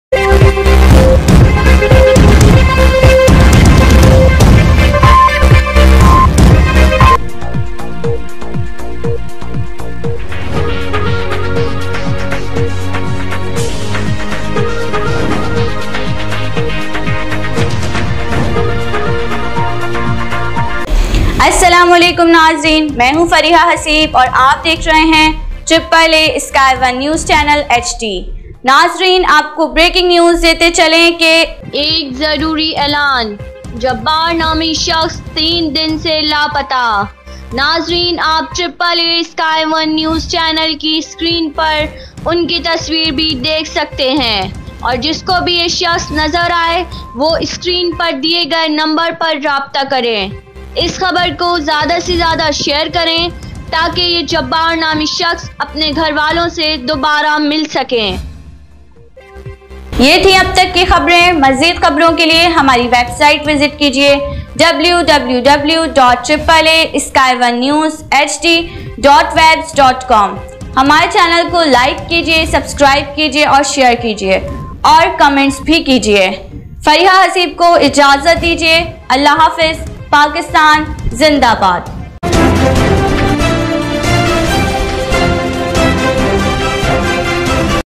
Breaking News ضروری اعلان جبار نامی شخص تین دن سے لاپتہ AAA SkyOne News HD 26-04-2021
tweeter :- @AAASkyonenewsH2

ضروری اعلان جبار نامی شخص تین دن سے لاپتہ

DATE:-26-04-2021